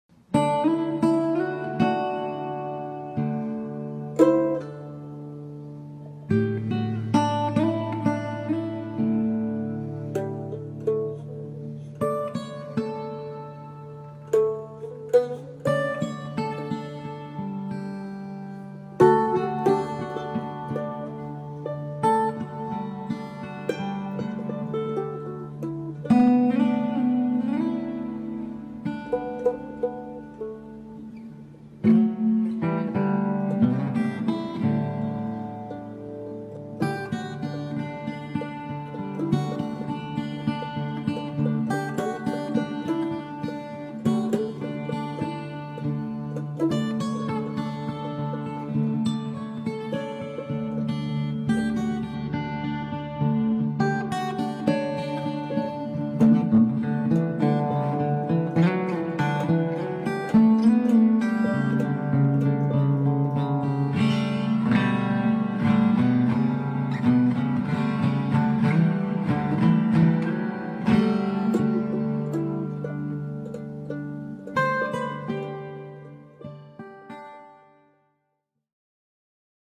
Soundscape improvisation #54 - piano improvisation
Soundscape improvisation #54
with a guitar track by Chris Hlavka
Artwork and photograph by Catherine Mommsen

This soundscape improvisation post is a bit of a unique one. I found this inspiring guitar meditation by Chris Hlavka, who kindly allows me to remix with my mandolin improvisation. I’m still a beginner mandolin learner but I couldn’t help trying this when I heard Chris’ such atmospheric improvisation.

So, with this post, music came first, then I thought of Catherine’s beautiful drawing to go with it.

Thank you Chris and Catherine for the inspiration!

Recorded on iPad, 16 February 2023